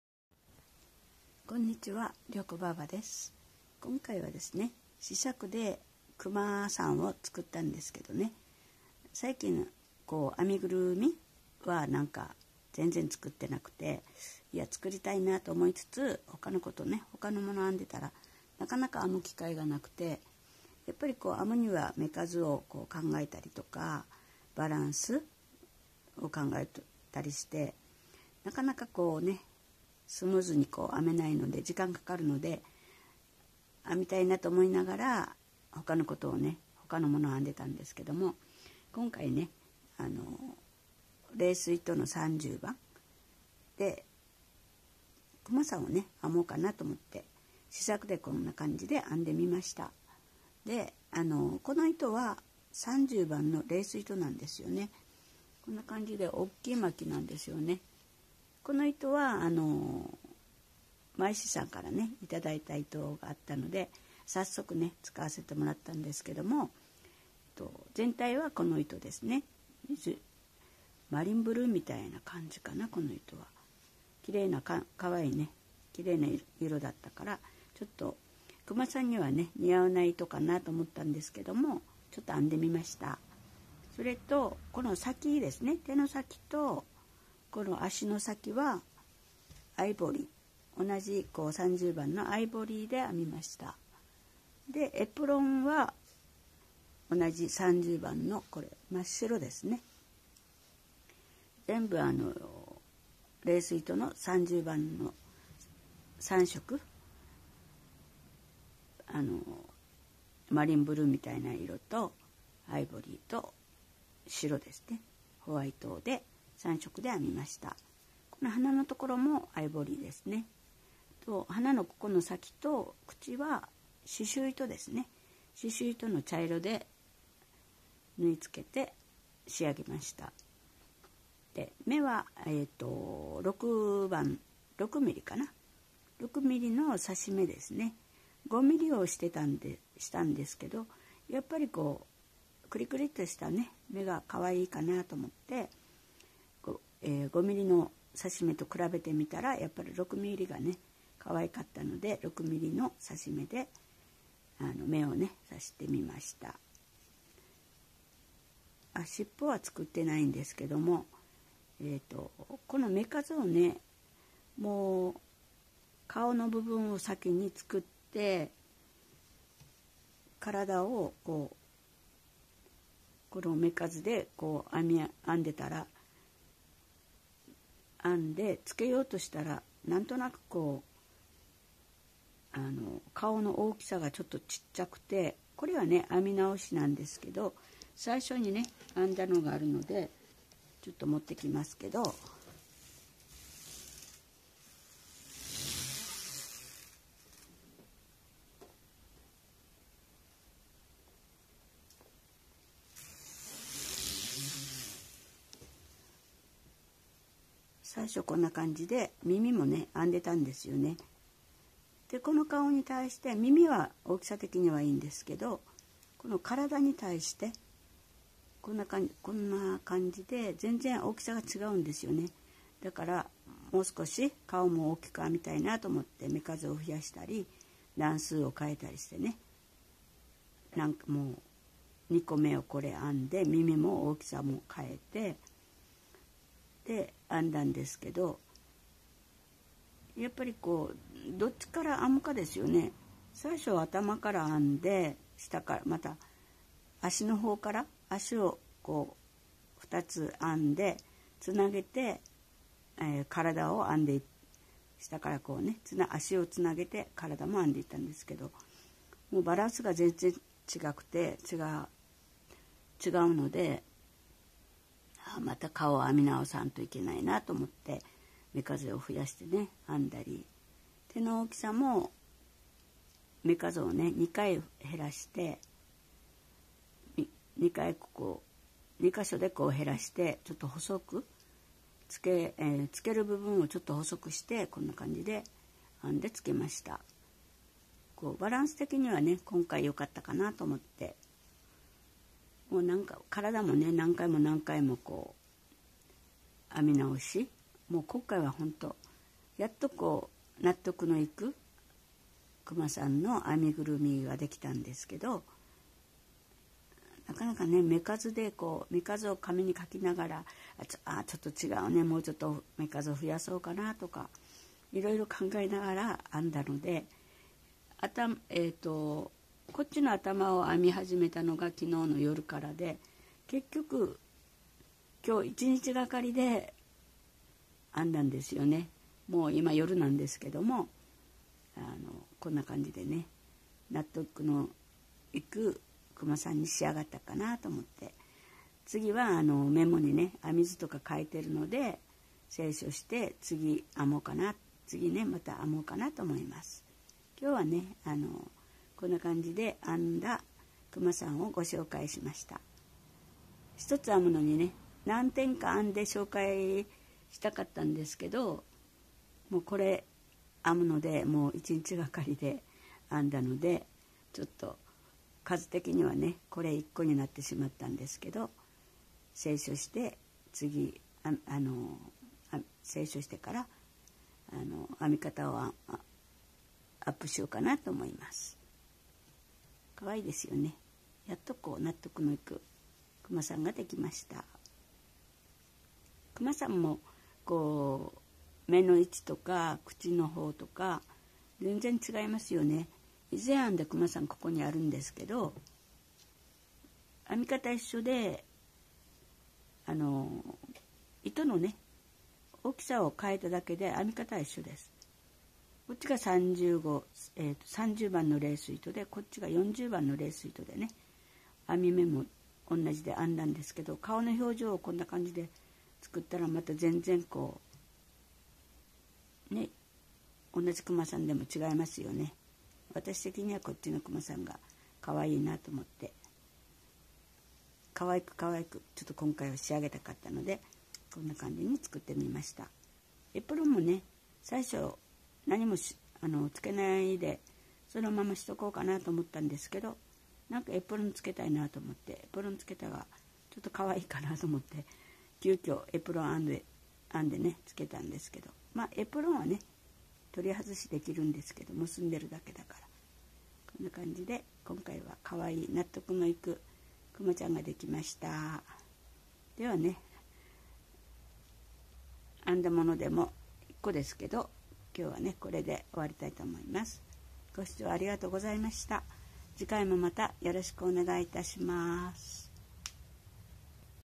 レース糸で試作のクマさん編みました。編み直しで一日掛かった
レース糸でクマさんの試作を編みました。
何度も編み直し。

メルカリフリマアプリ「メルカリ」でハンドメイドのんのんさんが販売中